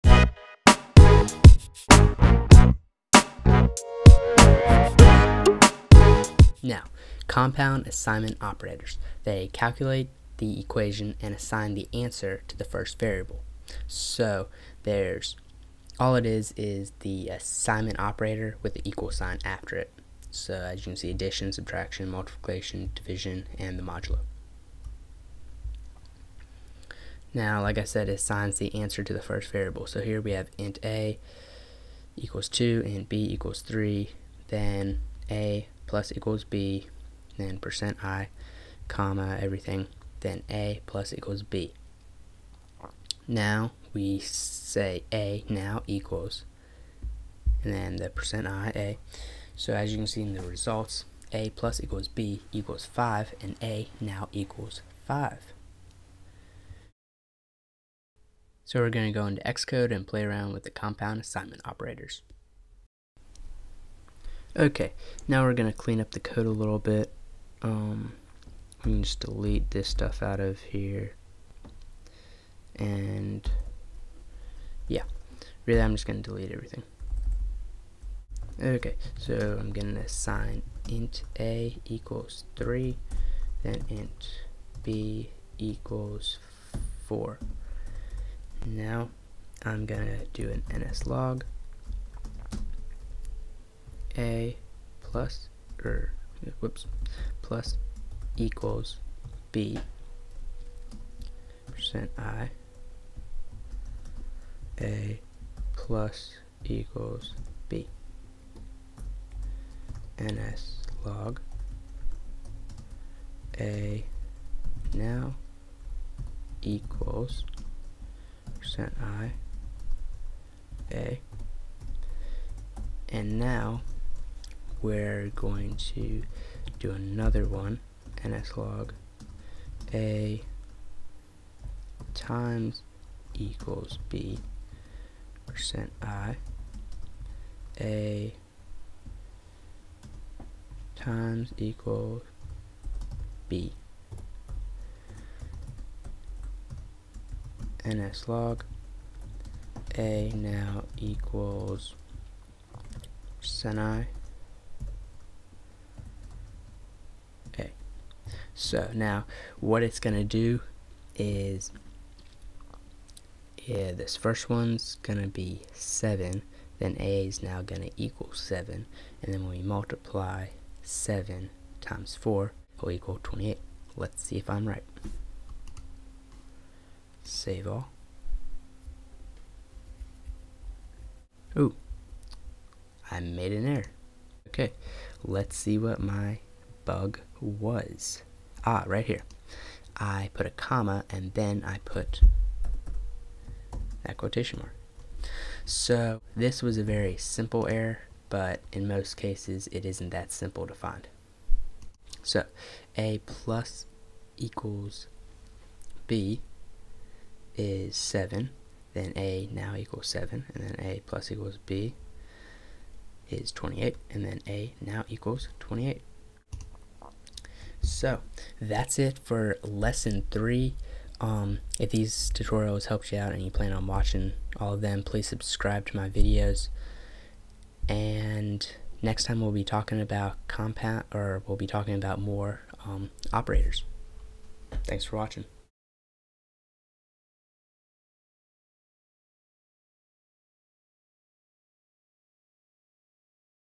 Objective-C Tutorial - Lesson 3: Part 2: Compound Assignment Operators
I show you what compound assignment operators are and how they can save time typing.

Objective-C Tutorial
objective-c tutorial
objective c tutorial
objective c programming tutorial
objective c programming tutorials